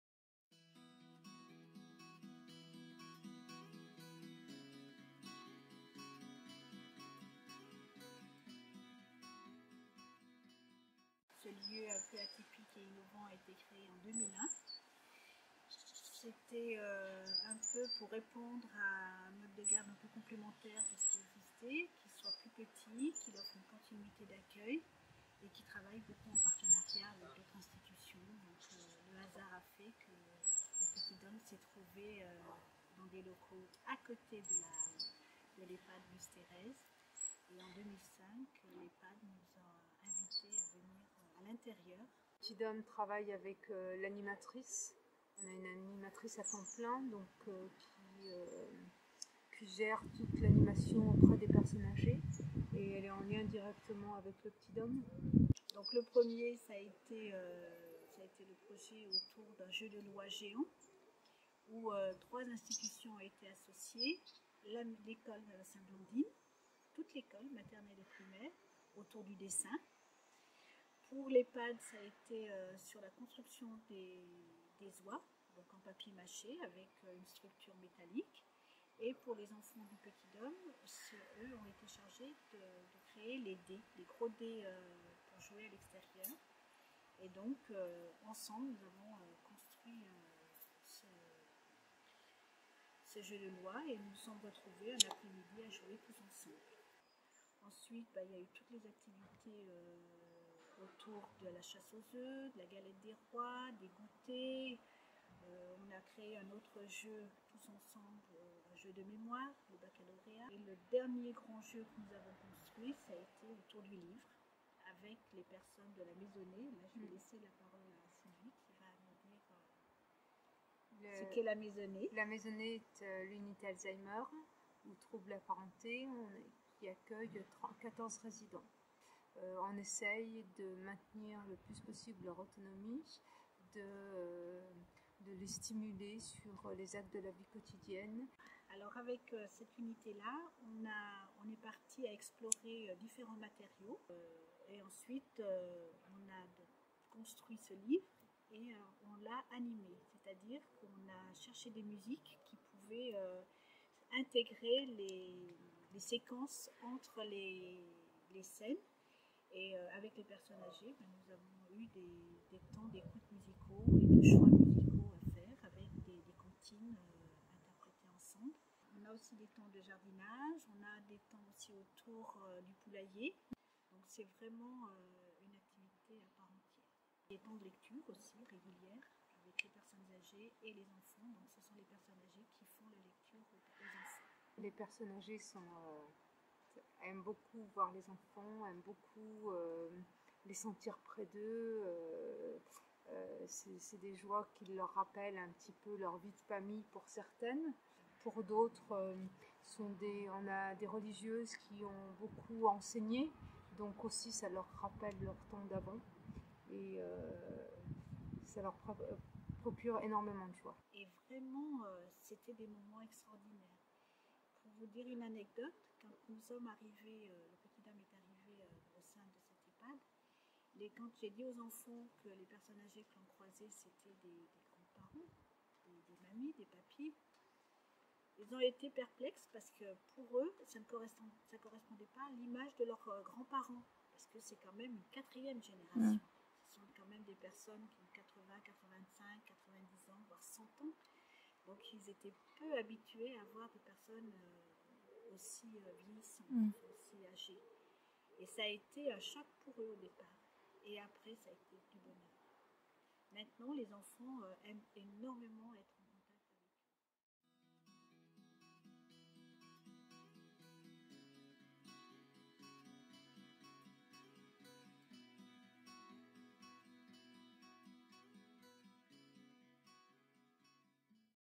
MAM Le Petit d'Homme (Ecully)
vidéo qui illustre le plaidoyer "Valoriser la petite-enfance avec l'Economie Sociale et Solidaire" septembre 2017